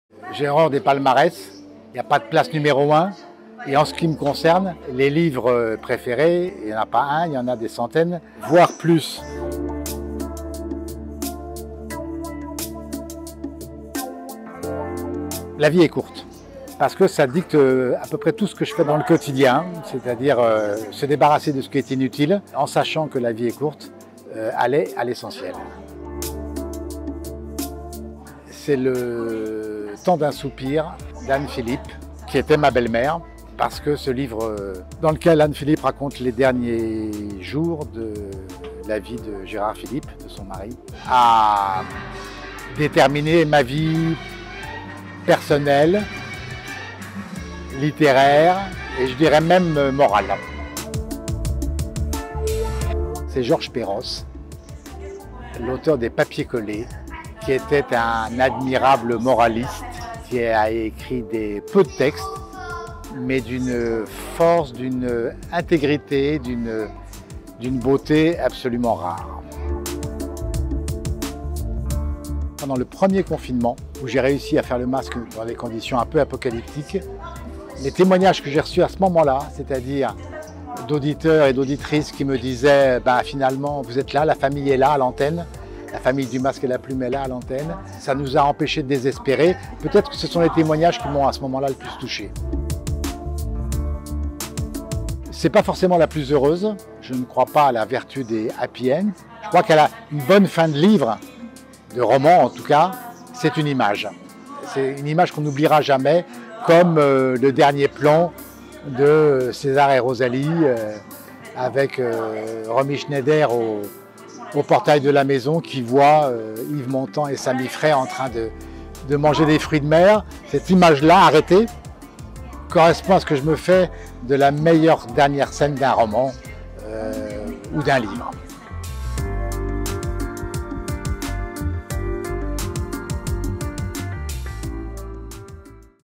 Dimanche 6 juin 2022 | Rencontre avec Jérôme Garcin
🎙️Rendez-vous culturel emblématique de @franceinter depuis plus de 50 ans, « Le masque et la plume » était en public ce week-end depuis le festival littéraire MOTpourMots. Retrouvez l'interview de Jérôme Garcin, présentateur de l'émission.